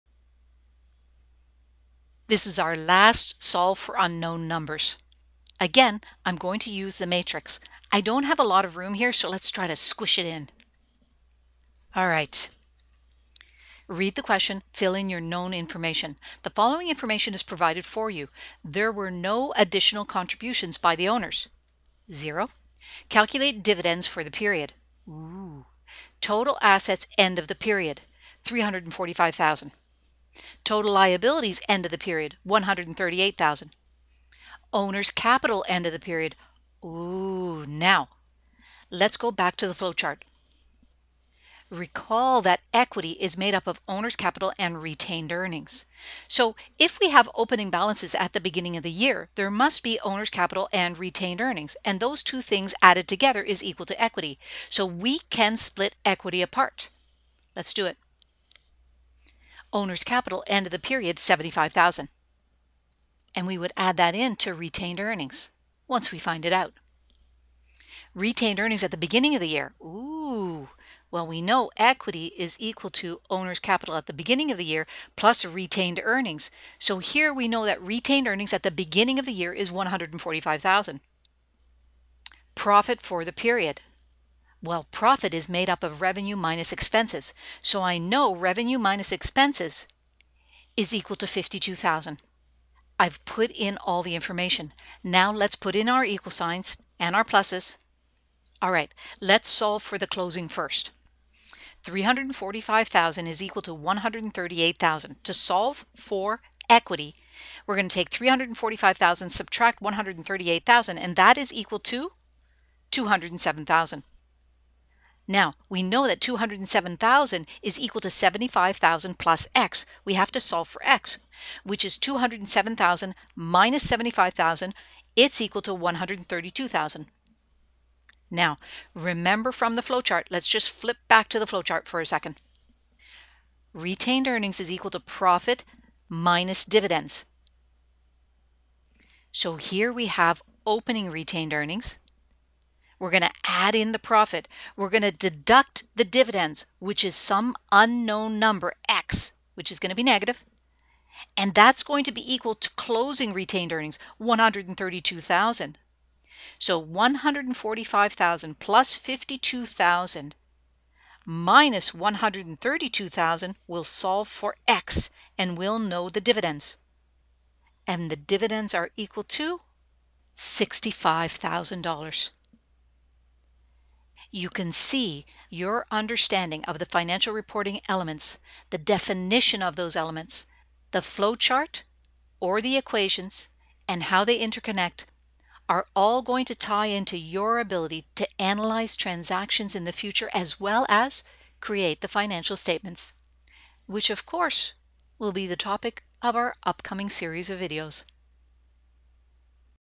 Module 1 - Solving for Unknown Numbers, Part 3
My last video showing how to use the expanded accounting equation in a flow chart to solve for unknown numbers.

Else